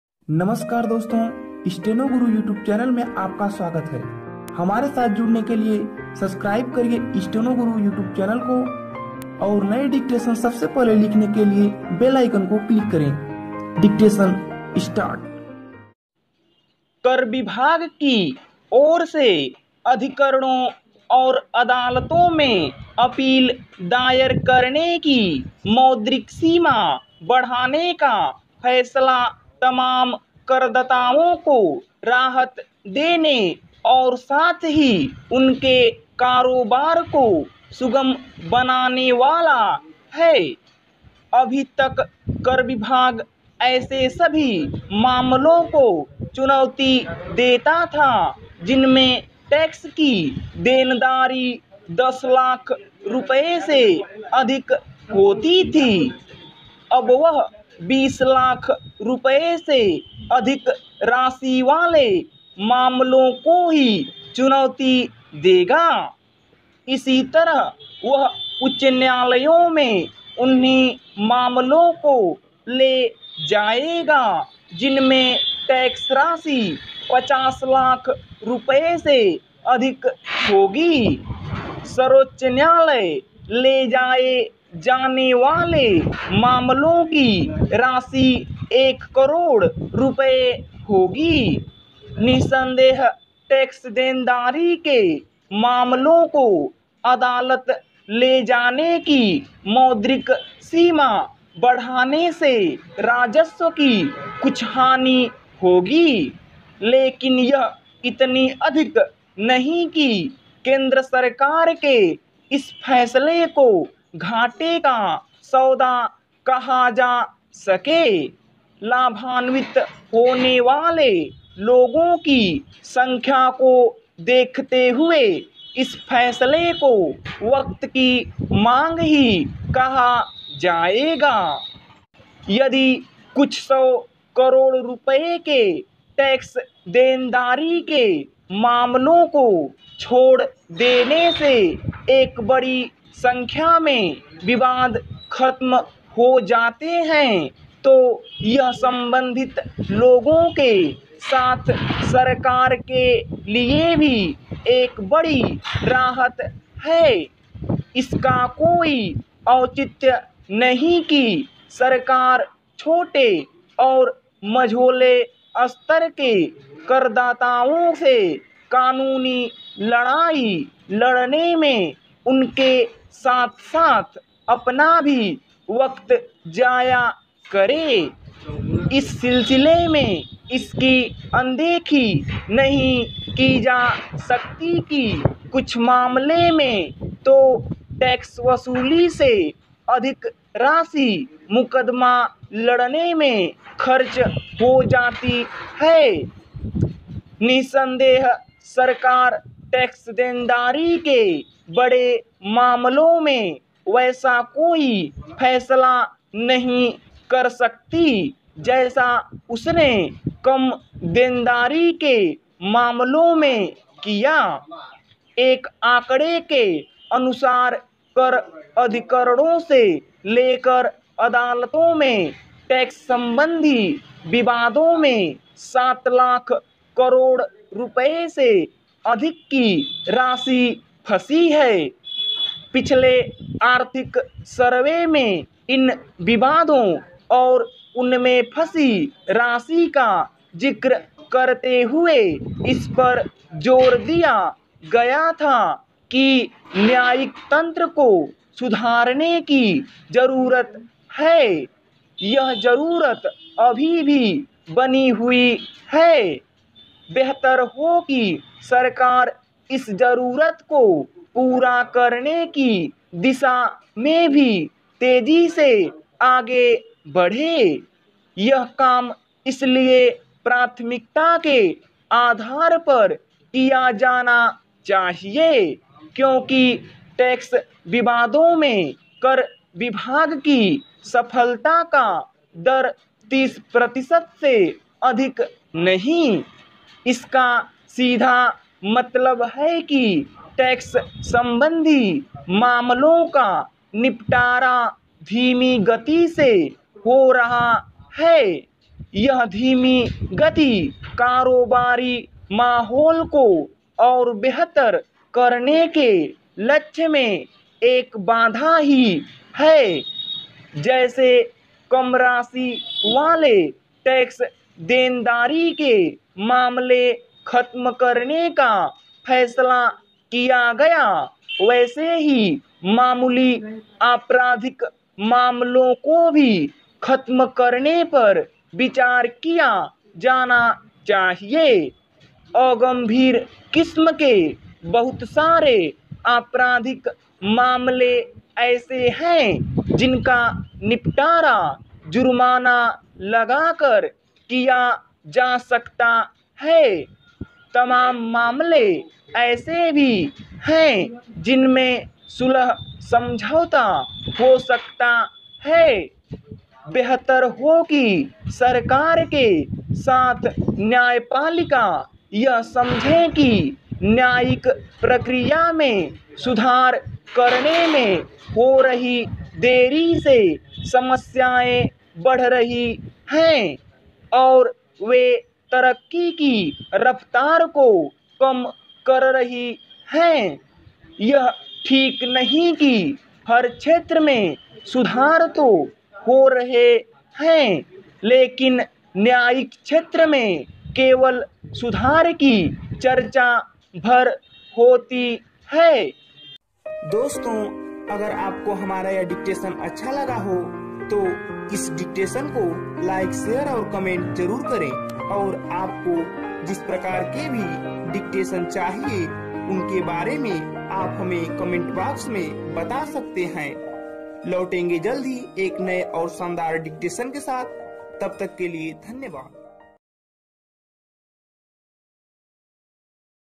13 जुलाई संपादकीय डिक्टेशन 80 शब्द प्रति मिनट 512 शब्द Sampadkiya Dictation || Steno Guru
13 जुलाई संपादकीय डिक्टेशन 80 शब्द प्रति मिनट 512 शब्द  Sampadkiya Dictation || Steno Guru ||

Thanks for writing our dictation